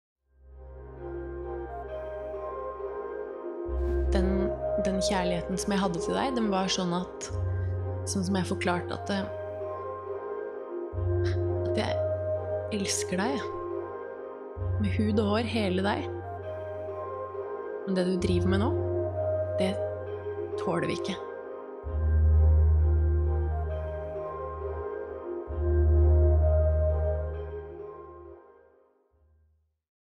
«Uten navn» teaser 2018
Stykket Uten navn – Å miste sine nærmeste, er produsert av Østfold kulturutvikling i samarbeid med Kulturdråpen, skrevet og utviklet av Fredrik Høyer og Mattis Herman Nyquist.